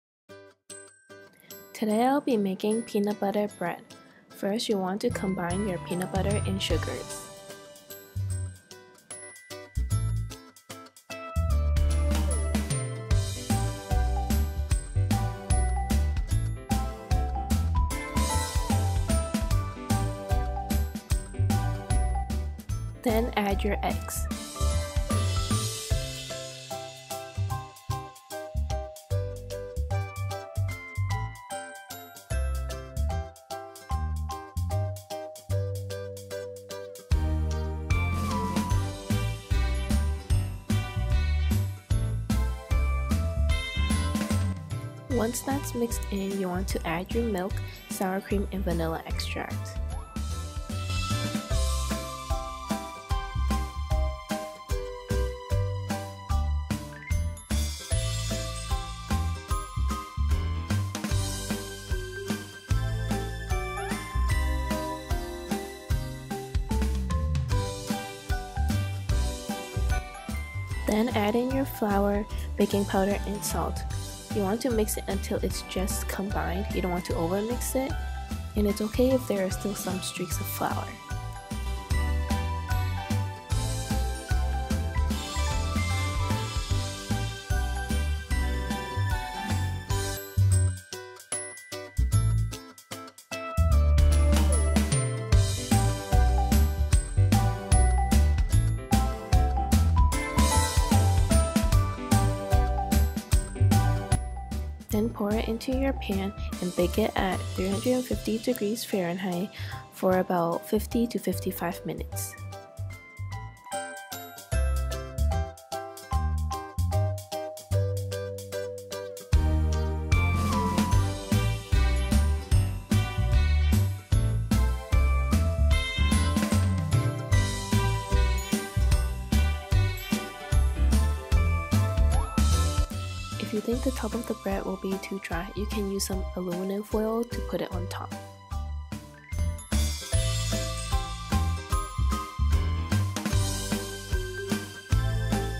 Peanut Butter Bread
Ingredients:
1 cup creamy peanut butter
1/3 cup granulated sugar
1/3 cup brown sugar
2 eggs
3/4 cup milk
1/2 cup sour cream
1 Tsp. vanilla extract
2 cups all purpose flour
1 Tbsp. baking powder
1/4 Tsp. salt